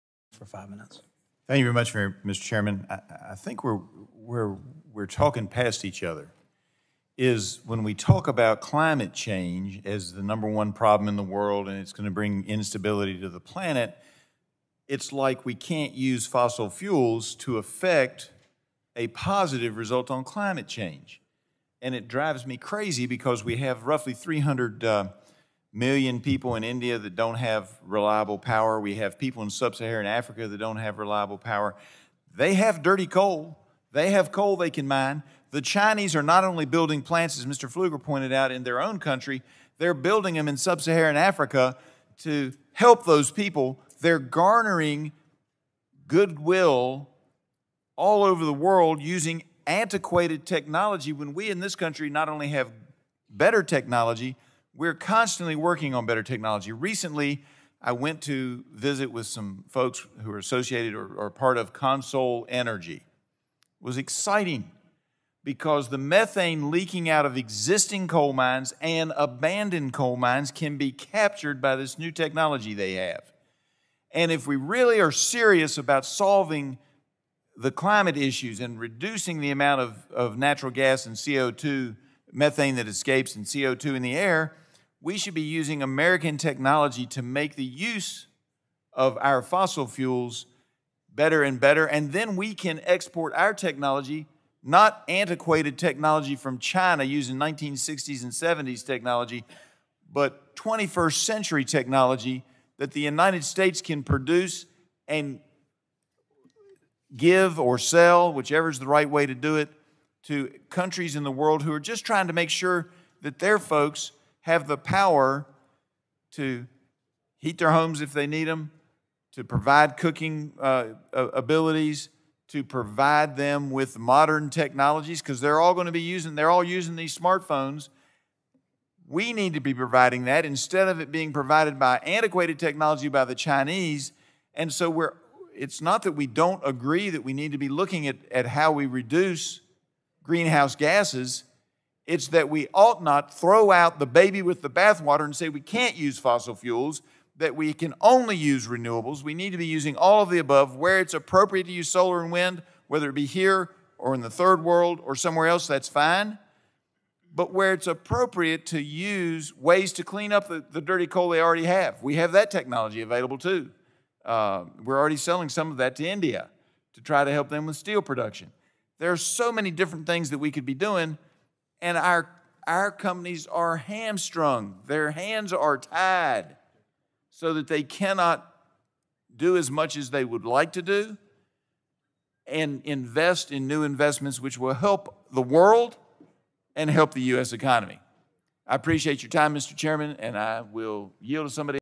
Griffith Provides Comments At Energy and Commerce Energy Subcommittee Markup
Congressman Morgan Griffith proudly represents the Ninth Congressional District of Virginia in the United States House of Representatives.